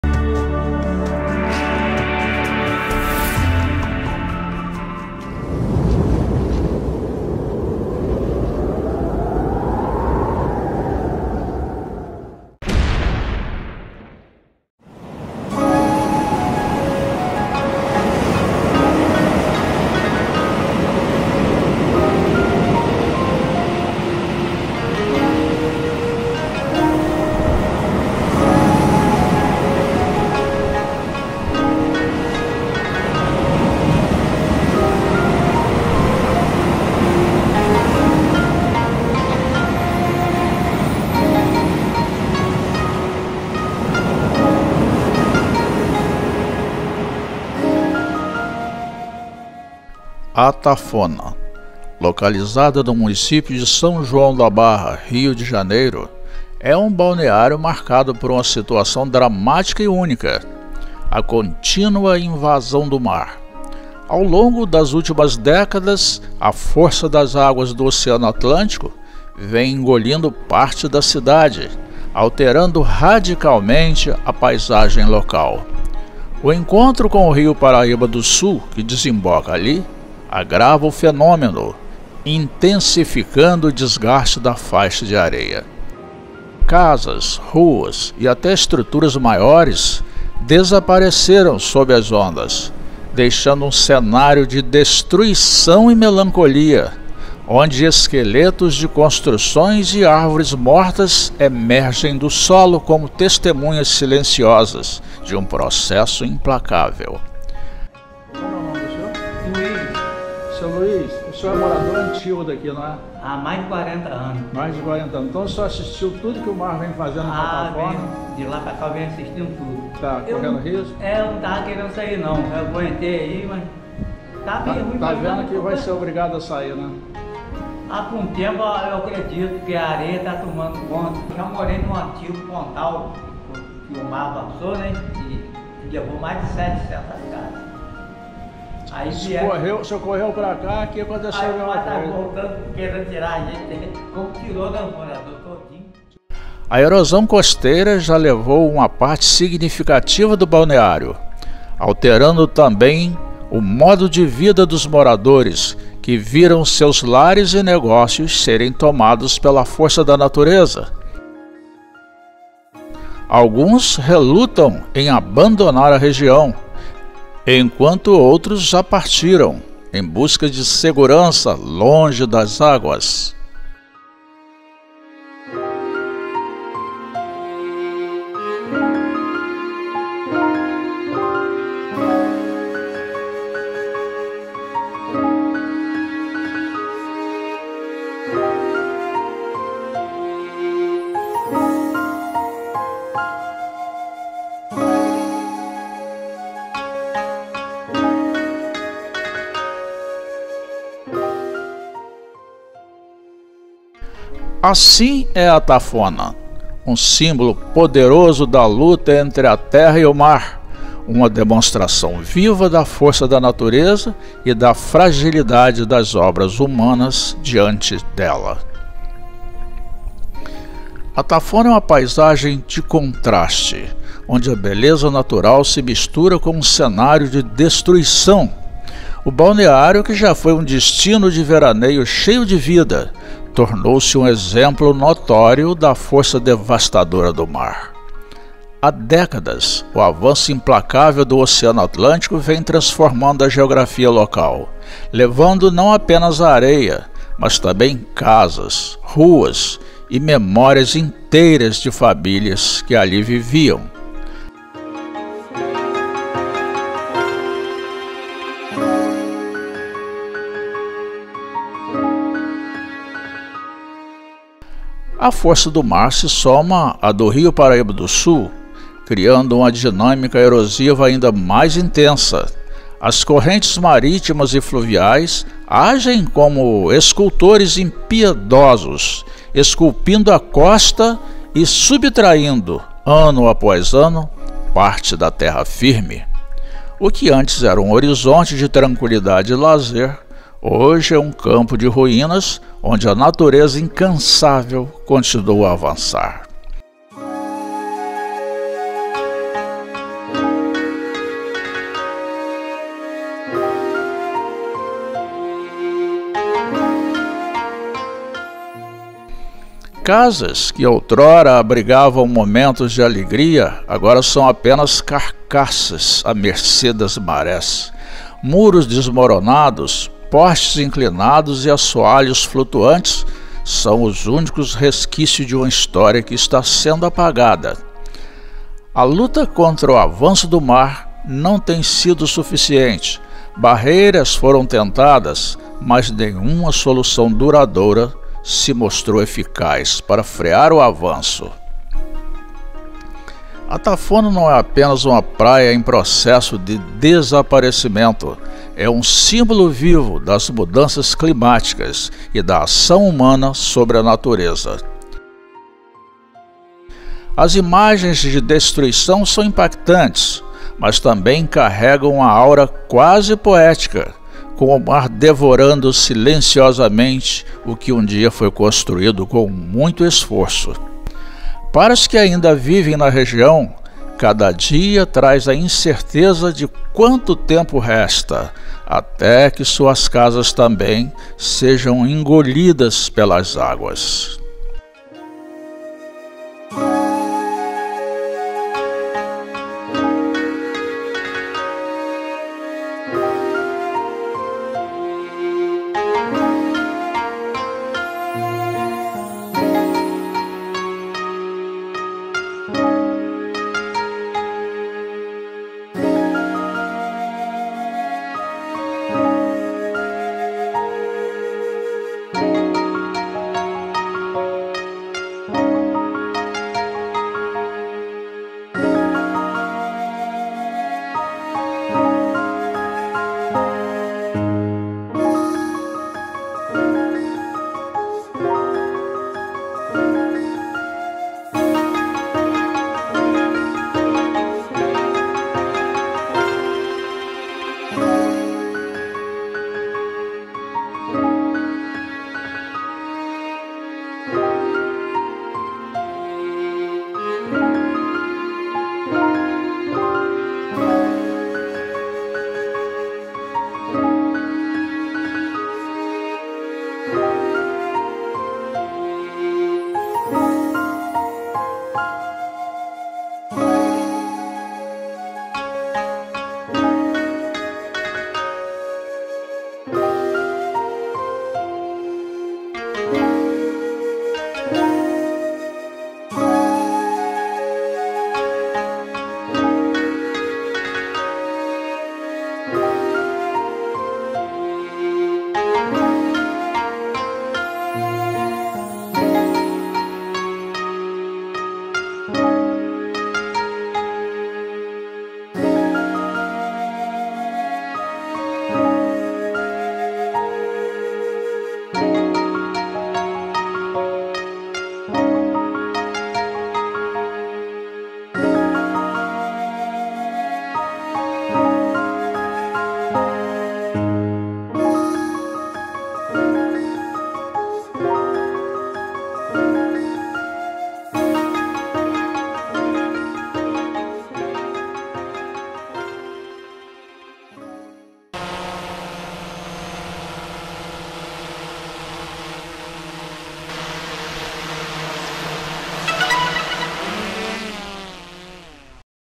ATAFONA - DESTRUIÇÃO À VISTA
Atafona, localizada no município de São João da Barra, Rio de Janeiro, é um balneário marcado por uma situação dramática e única: a contínua invasão do mar. Ao longo das últimas décadas, a força das águas do Oceano Atlântico vem engolindo parte da cidade, alterando radicalmente a paisagem local. O encontro com o Rio Paraíba do Sul, que desemboca ali, agrava o fenômeno, intensificando o desgaste da faixa de areia.

Roteiro, texto, imagens, edição e narração: Pereirinha